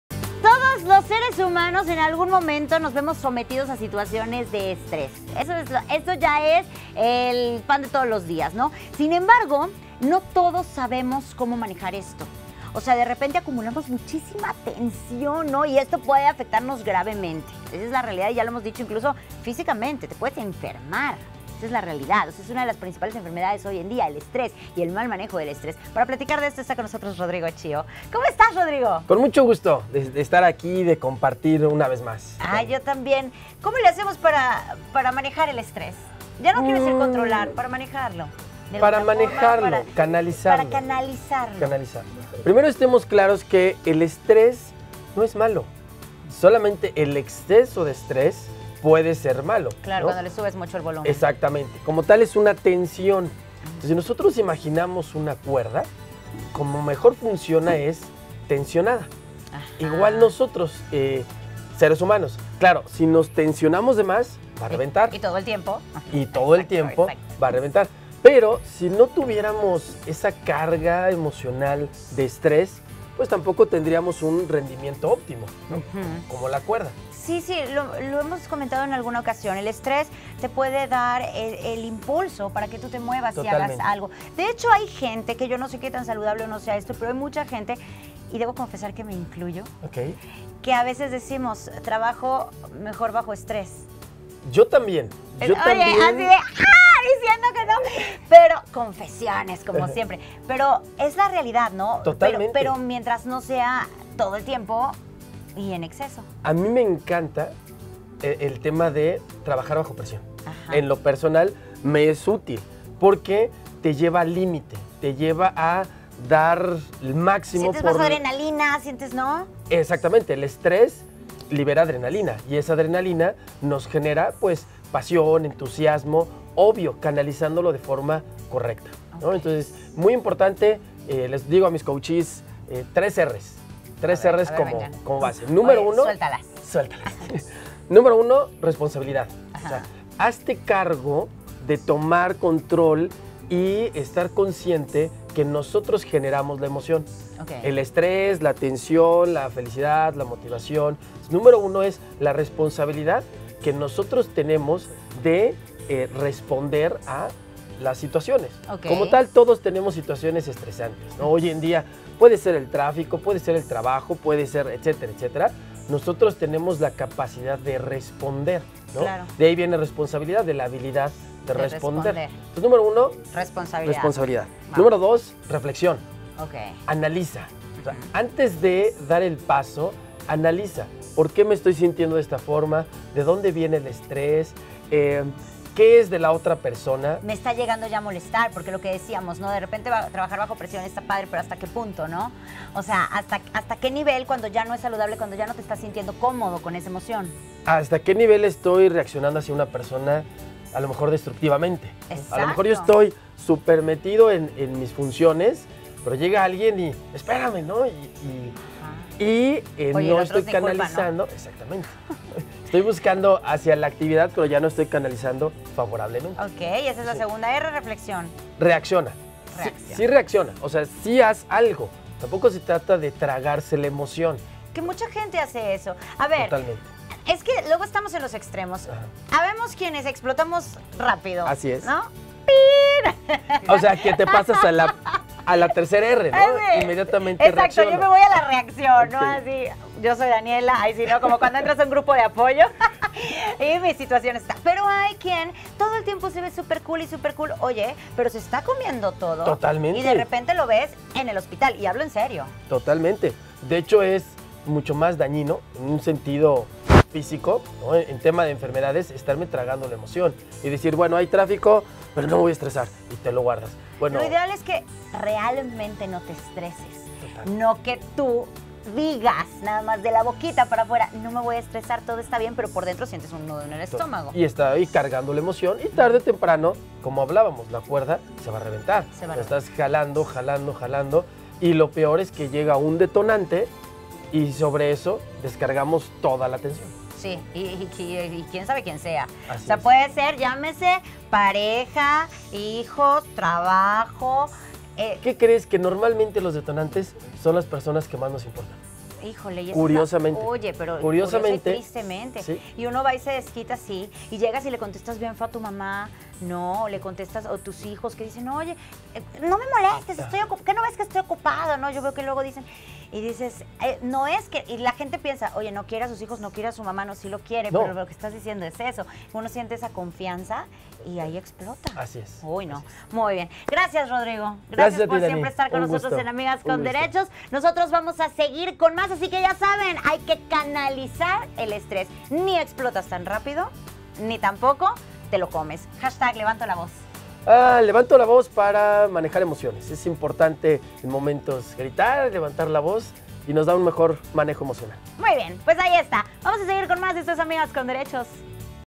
¿Cómo canalizar el estrés?.
El estrés es perjudicial para la salud física y emocional. Para RodrigoChío el estrés no es malo, lo que nos daña es el exceso de estrés, pues finalmente es una tensión, que como una cuerda, tarde o temprano se rompe. ¡Escucha con más detalle su punto de vista sobre este tema!.